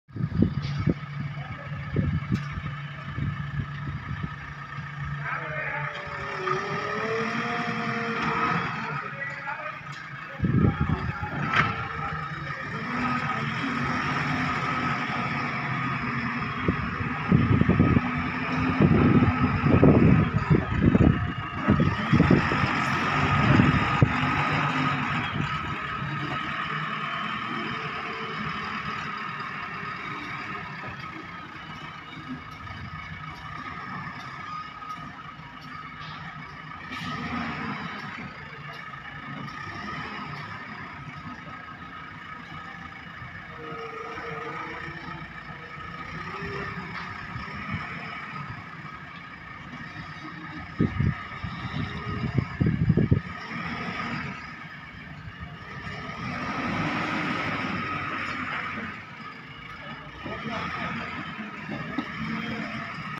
JCB backhoe loader leveling work
JCB backhoe loader new bs4 engine bed leveling work
New JCB machine working with loader